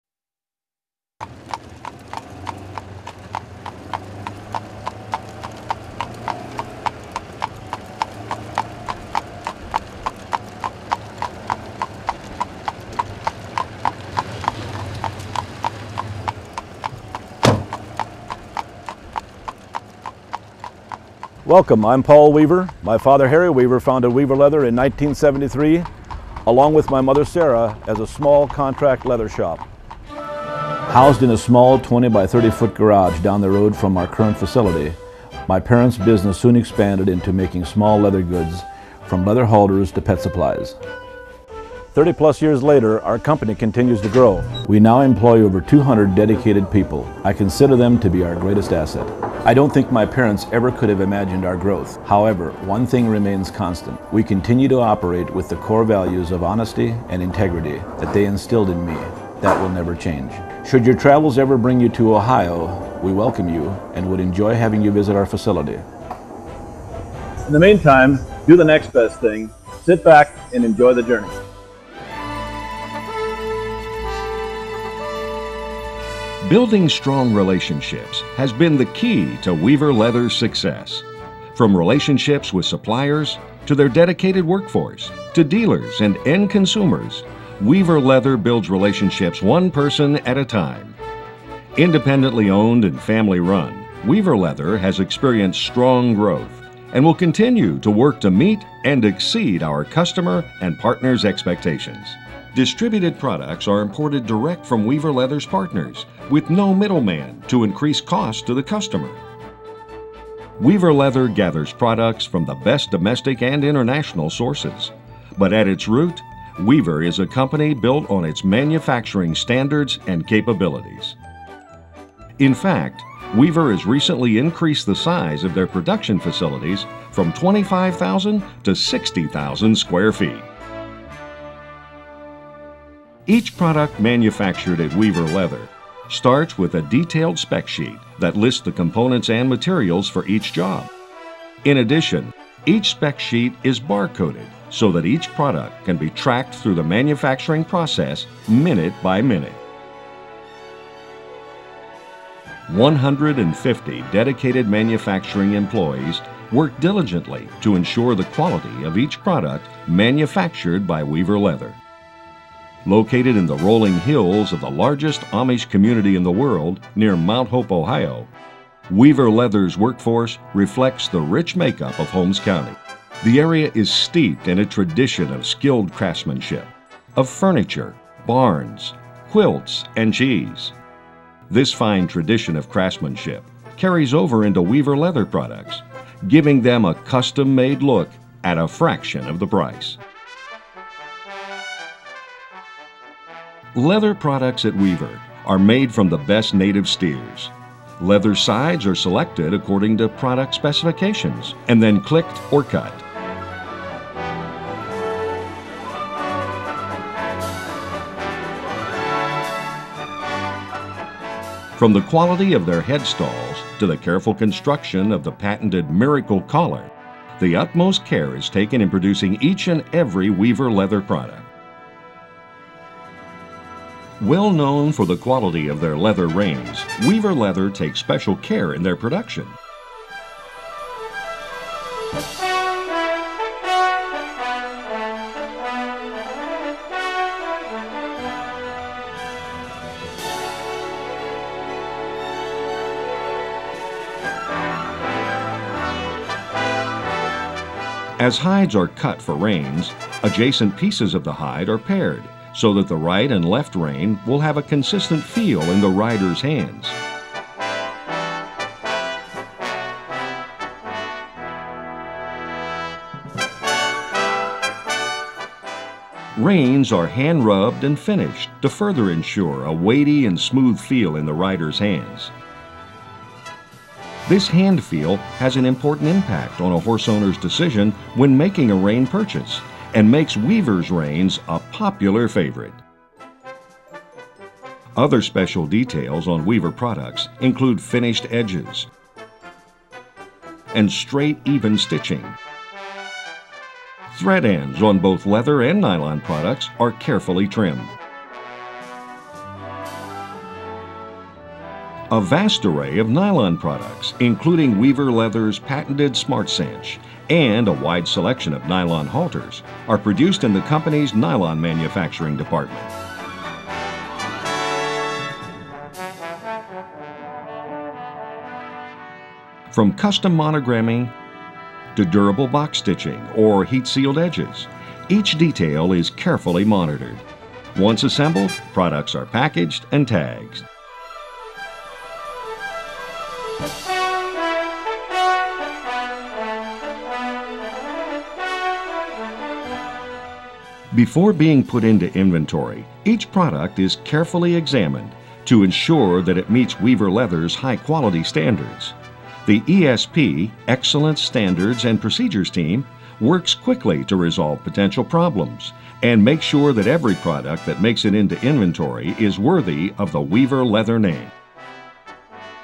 Weaver Leather part 1
The Weaver Leather Company may be the world's largest provider of equine leather products.  It's located in Mount Hope, Ohio in the heart of Ohio's Amish Country.  By watching this video, you'll learn the company's history and watch Amish craftsmen and women at work.